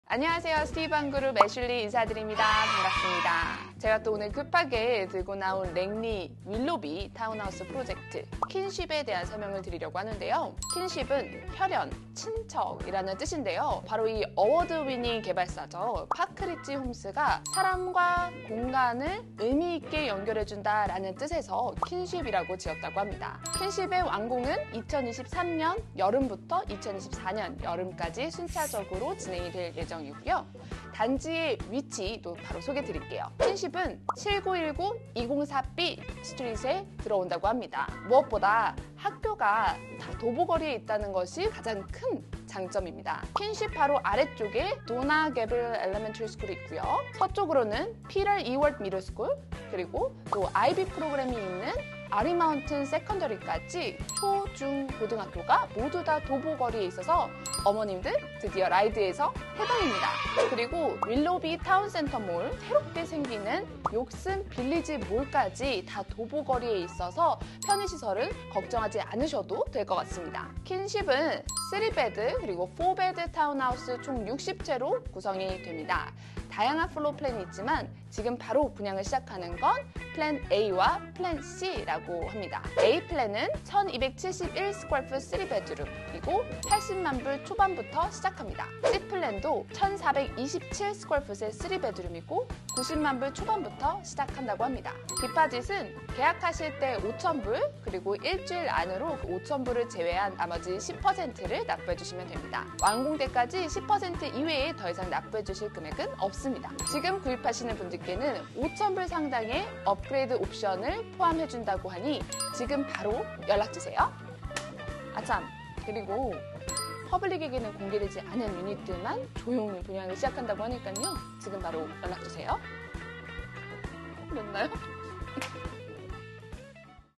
랭리 윌로비 타운하우스 선착순 분양!! KINSHIP | 스티브한 밴쿠버 부동산 TV
안녕하세요.
스티브한부동산TV입니다.

오늘영상은
랭리 윌로비 타운하우스 프로젝트인 KINSHIP에 관한 분양정보 영상입니다.
방송에서 자세히 말씀 못드린 정보도 가지고 있으니
관심있으신 분은 아래 연락처로 신속히 연락 부탁드립니다!
그리고 선착순 분양 입니다!

감사합니다.